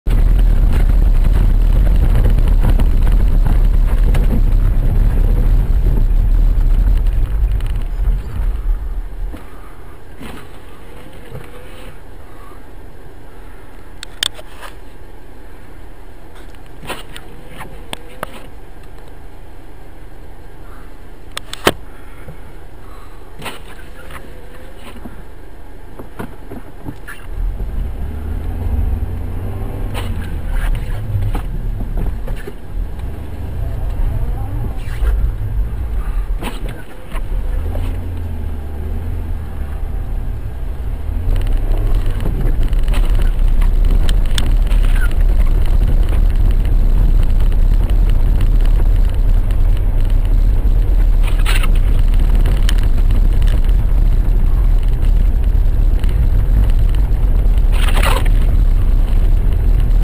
Занесло машину на трассе М8
Трасса М8 населенный пункт Шилово (Архангельская область).
Занесло на зимней дороге